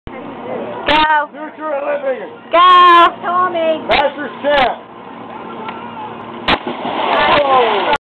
Brads Famous Bellysmackers
Brad has won competitions with this shocking jump